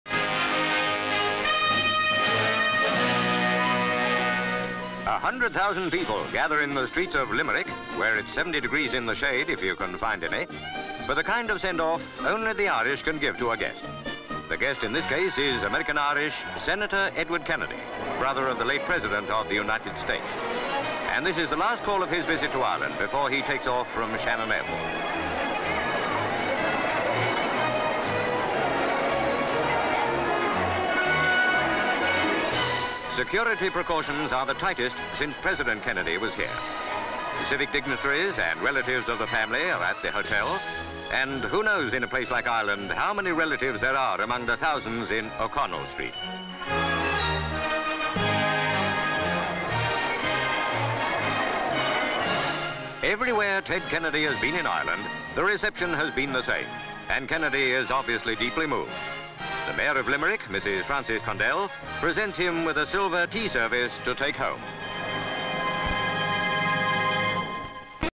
Ted Kennedy In Ireland (1964)
Full title reads: "Limerick. Ted Kennedy In Ireland".

Limerick, Eire, Southern Ireland, Republic of Ireland. Senator Edward Kennedy pays a visit to his family's historic homeland.

GV Crowds gathered in street to welcome Senator Kennedy to Limerick. GV Crowds watching Irish dancers. SV Girls dancing. SV Crowd waving US American flags. LV Senator Teddy Kennedy arrives standing in open car as the car stops the crowds surge around him. CU Small boy waving Stars and Stripes flag. SV Kennedy steps from car. CU As he makes his way through crowd. GV The special platform outside the Cruises's Hotel with guests on it. GV Pan mass crowds in O'Connell Street. Pan to SV of Kennedy waving. CU Kennedy waving. GV Mass crowds waving. CU Mr Kennedy speaking. SV Official guests on platform. CU The Mayor of Limerick Mrs Frances Condell presents Senator Kennedy with a silver tea service.

(Orig.Neg.)
 FILM ID:1761.18